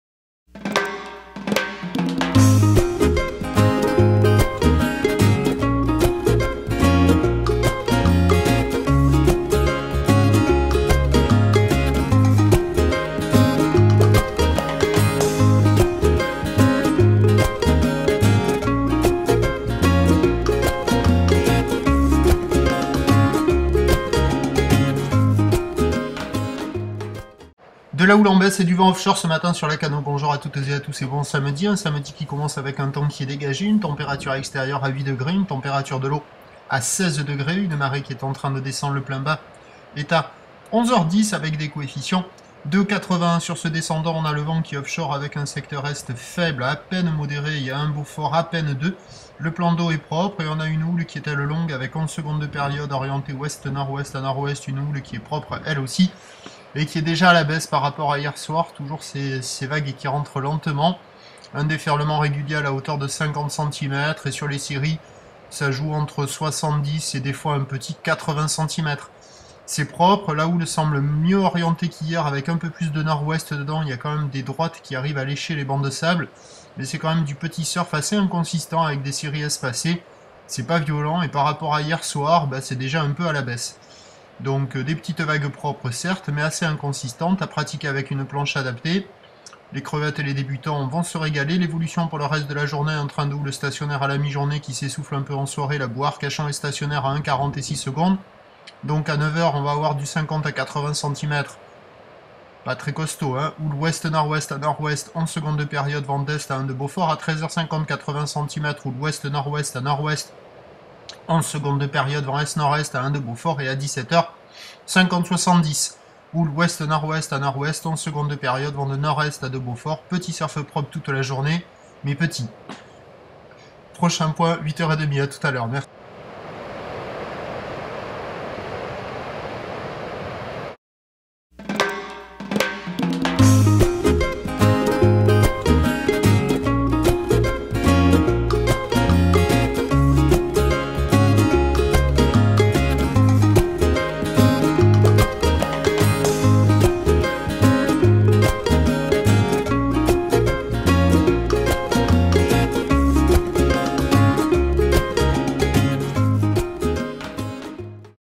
Lacanau Surf Report Vidéo - Samedi 29 Octobre 8H
Houle longue WNW-NW en baisse, vent offshore faible à modéré.